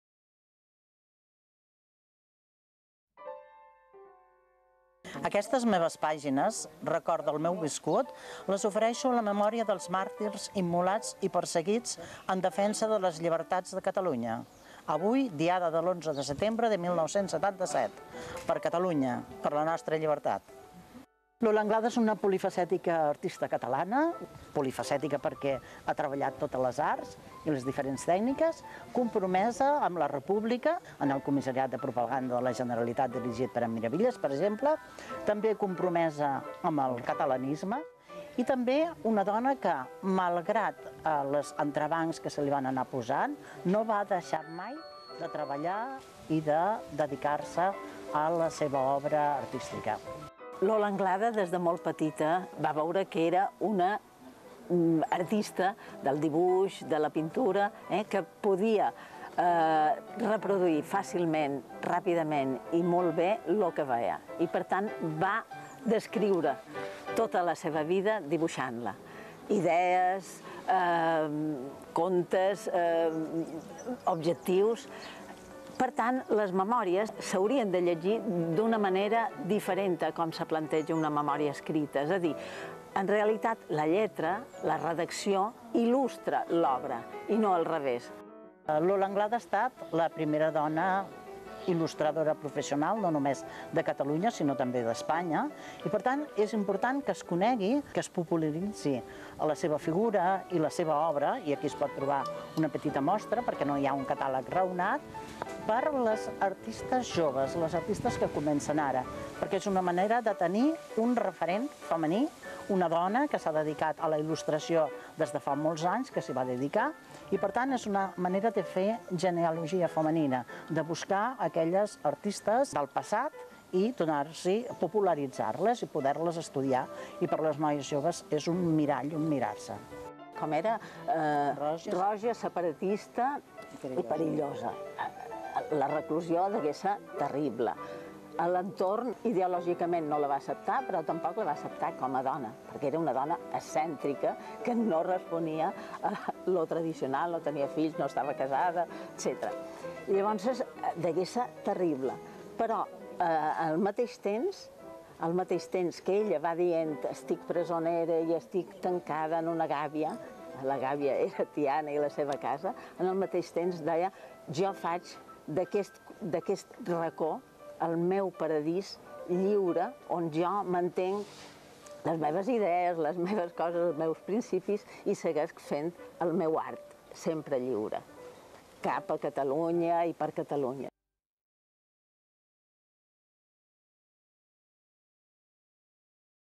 'Lola Anglada. Memòries 1892-1984'
Lola Anglada va ser la il·lustradora catalana més prolífica de la primera meitat del segle XX i la primer dibuixant professional de Catalunya i d'Espanya. Artista polifacètica, a banda de dibuixant il·lustradora també va ser escriptora, decoradora, pintora, ceramista i escultora.

Abans de morir, va preparar un conjunt d'escrits, poemes, dibuixos, cartes… que volia publicar a manera de memòries. La Diputació de Barcelona les ha editat a cura de Núria Rius i Teresa Sanz, que han realitzat la tasca d'endreçar tot aquest material i explicar, amb notes a peu de pàgina, els esdeveniments socials, llocs o simplement mots que esmenta l'autora.